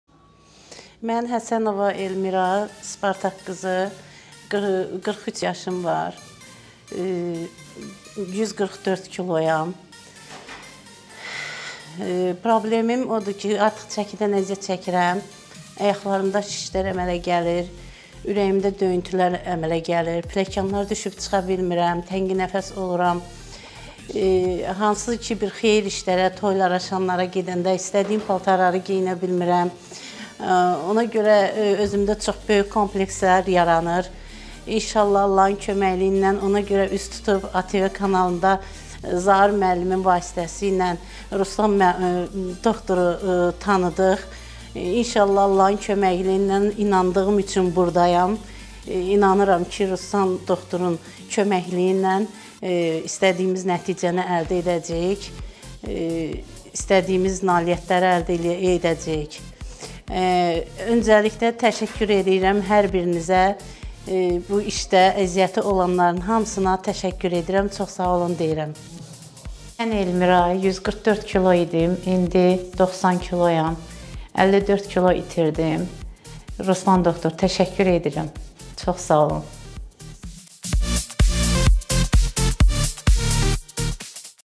7 ay ərzində itirilən 54kq !!!
Ümumi-bariatrik cərrahımız -dr.Ağayev Ruslanın sayəsində  bir xanımımız daha komplekslərindən azad olur!👏
Sadəcə 7 ay ərzində itirilən 54kq çəki...
Ps.: Bu hələ son nəticəmiz deyil.😊

Op. Dr. Ağayev Ruslan
Ümumi-bariatrik cərrah